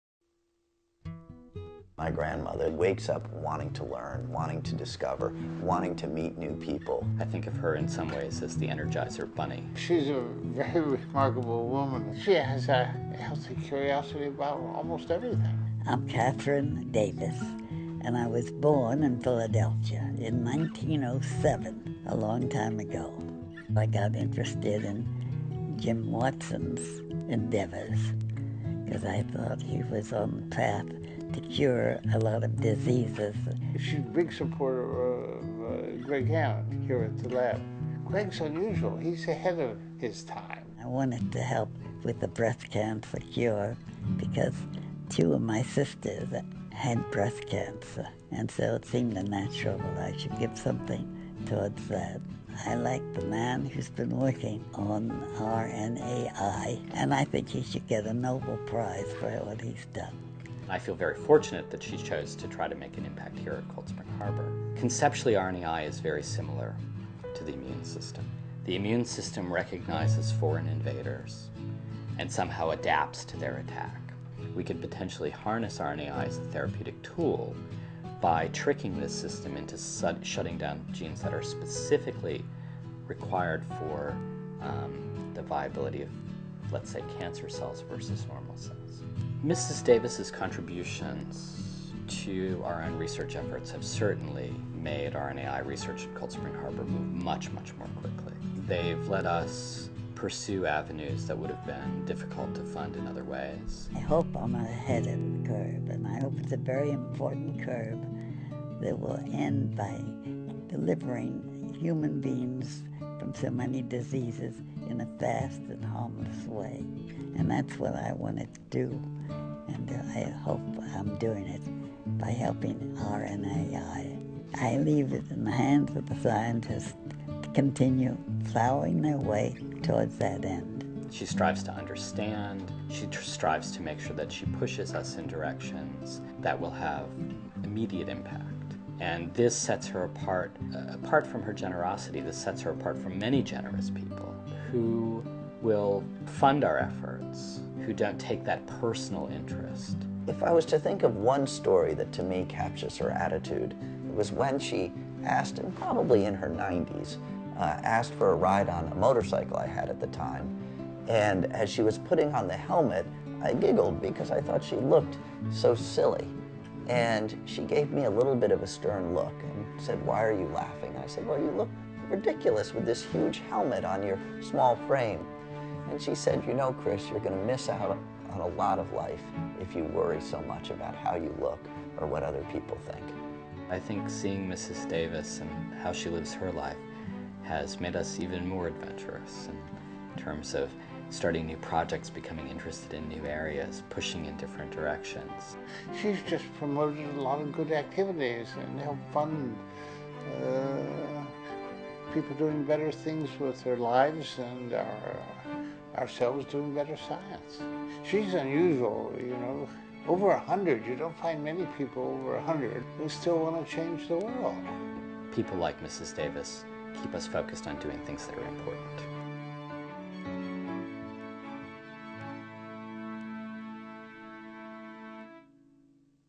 Double Helix Medals Dinner - Kathryn W. Davis
Kathryn Wasserman Davis is a 102-year-young example of genetics at its best. A lifelong philanthropist and advocate for research, Mrs. Davis and her family established the Davis Chair in Human Genetics at Cold Spring Harbor Laboratory to focus on uncovering the roots of genetic disorders. She also founded the Kathryn W. Davis RNAi Research Center at Cold Spring Harbor to support the understanding of how this cellular machinery might be programmed to turn off genes that lead to cancer and other disorders.

In addition to her commitment to biomedical research, Mrs. Davis is a noted supporter of the arts, education, conservation, global peace initiatives, and representative government. She has nurtured a lifelong passion for cross-cultural education, international relations, and the Soviet Union and Russia.

Mrs. Davis has lectured extensively on Russia and the Soviet Union, written about foreign affairs for numerous periodicals, and is a member of the Council on Foreign Relations. The Woodrow Wilson Center for International Scholars presented her with its Woodrow Wilson Award for Public Service, honoring individuals who have served with distinction in public life.

Through the foundation established by her late husband, Shelby Cullom Davis, a noted investment banker and U.S. Ambassador to Switzerland, Mrs. Davis fosters informed opinions on the world stage. Countless students at the Davis Center for Russian and Eurasian Studies at Harvard; the Davis Institute for International Studies at the Heritage Foundation; the Kathryn W. Davis Center for International and Regional Studies at the College of the Atlantic; Wellesley College; and a number of other colleges and universities have benefited from her generosity and entered the international arena prepared to make a difference in the world. Other organizations that benefit from Mrs. Davis' support are the East-West Institute, NARSAD and Scenic Hudson.

On her 100th birthday, Mrs. Davis founded Projects for Peace, which supports more than 100 peace initiatives each year undertaken by college students worldwide, and challenges future generations "to bring about a mindset of preparing for peace, instead of preparing for war."

Mrs. Davis holds a B.A. in Russian history from Wellesley, an M.A. in history from Columbia, and a Ph.D. in political science from the University of Geneva. Her doctoral thesis, "The Soviets at Geneva," focused on the Soviet Union's relationship with the League of Nations and was published in 1934 -- on the day the USSR joined the organization.